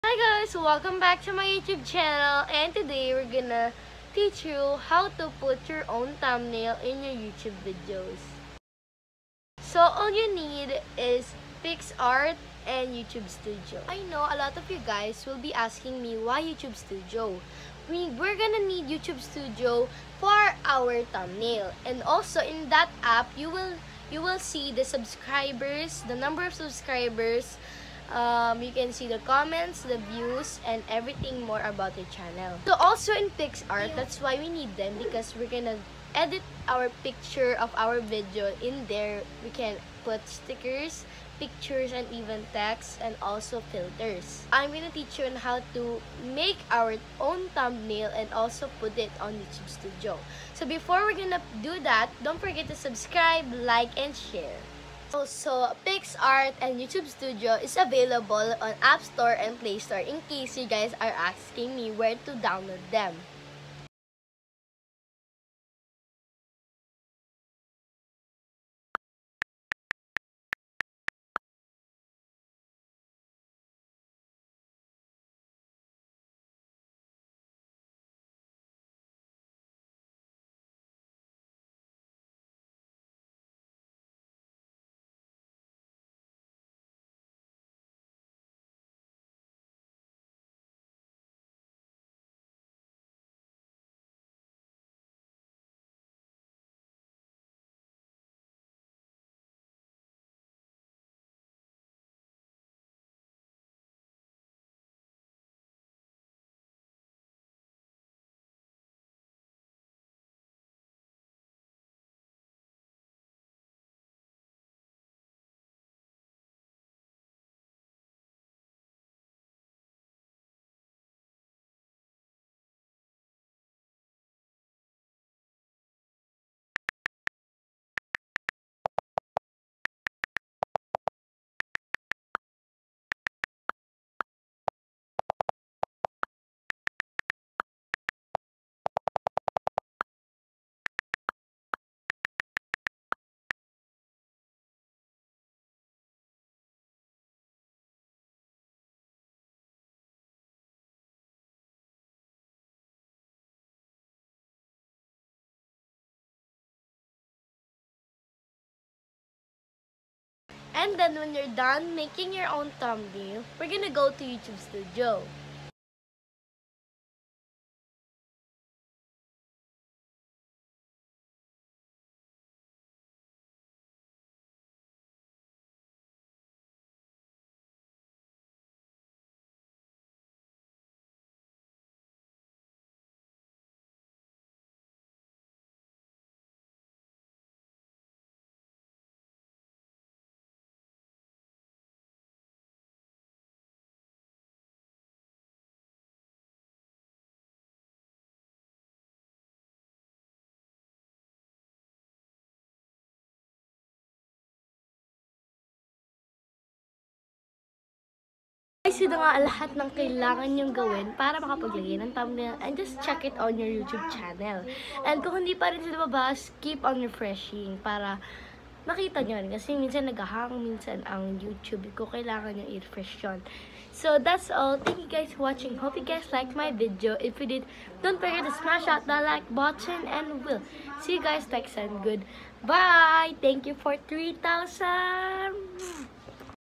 How To Put Ur Own ThumbNail For Youtube!! / Maya Javier
Today im showing how to put ur own thumbnail! Thank you so much for the support! I got accepted to work and get monitize by youtube! If it wasnt of u guys im not here! Thank you so much!